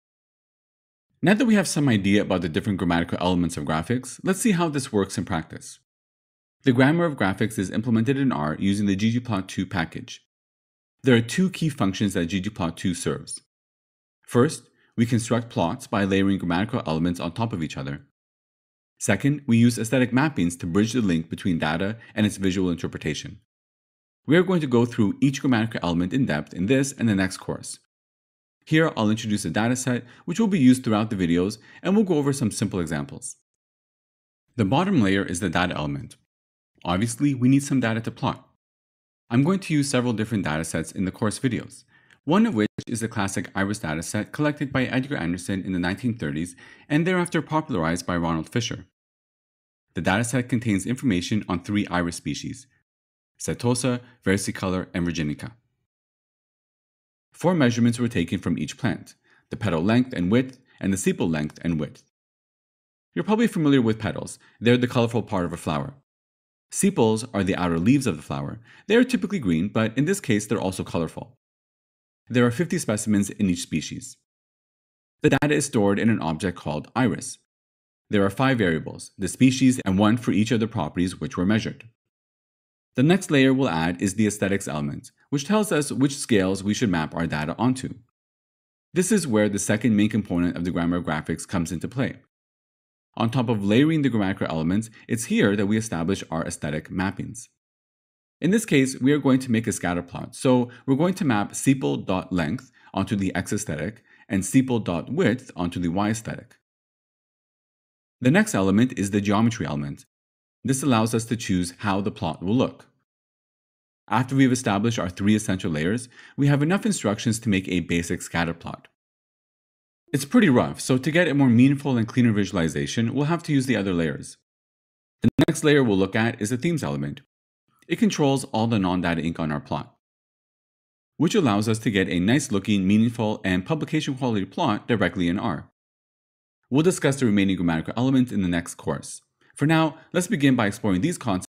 R Tutorial: ggplot2 layers
Want to learn more? at your own pace. More than a video, you'll learn hands-on coding & quickly apply skills to your daily work.

Now that we have some idea about the different grammatical elements of graphics, let's see how this works in practice.

The grammar of the graphic is implemented in R using the ggplot2 package.
There are two key functions that ggplot2 serves.

First, we construct plots by layering grammatical elements on top of each other.

Second, we use aesthetic mappings to bridge the link between data and it's visual interpretation.

We are going to go through each grammatical element in depth in this and the next course. Here I'll introduce a data set that will be used throughout the videos and we'll go over some simple examples.

The bottom layer is the data element. Obviously, we need some data to plot. I'm going to use several different data sets in the course videos,

One of which is the classic iris data set collected by Edgar Anderson in the 1930s and thereafter popularized by Ronald Fisher.

The data set contains information on three iris species, setosa, versicolor, and virginica.

Four measurements were taken from each plant - the petal length and width and the sepal length and width. You're probably familiar with petals, they're the colorful part of a flower. Sepals are the outer leaves of the flower, they are typically green, but in this case, they're also colorful. There are 50 specimens of each species.

The data is stored in an object called iris, there are five variables: the species and one for each of the properties which were measured.

In this case, we are going to make a scatter plot so we're going to map Sepal-dot-Length onto the X aesthetic and Sepal-dot-Width onto the Y aesthetic.

The next element is the geometry element. This allows us to choose how the plot will look.

After we've established our three essential layers, we have enough instructions to make a basic scatter plot. It's pretty rough, so to get a more meaningful and cleaner visualization, we'll have to use the other layers.

The next layer we'll look at is the theme's element. It controls all the non-data ink on our plot.

Which allows us to get a nice looking, meaningful and publication-quality plot directly in R.

We'll discuss the remaining grammatical elements in the next course.
For now, let's begin by exploring these concepts in the exercises.